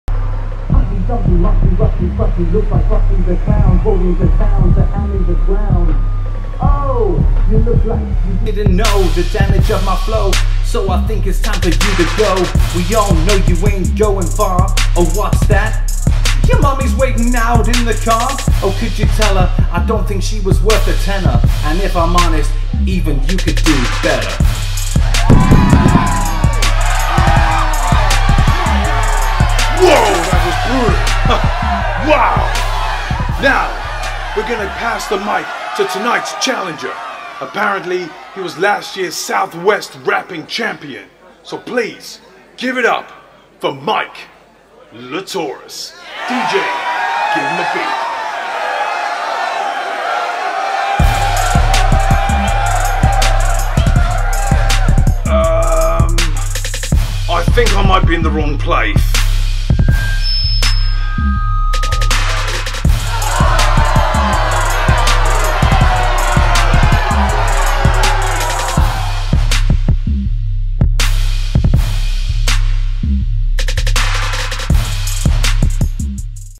Epic Freestyle Rap Battle
We visit the London underground rap scene... well kind of. its open mic night, lets see how this epic freestyle rap battle goes down

Headjunk Socials -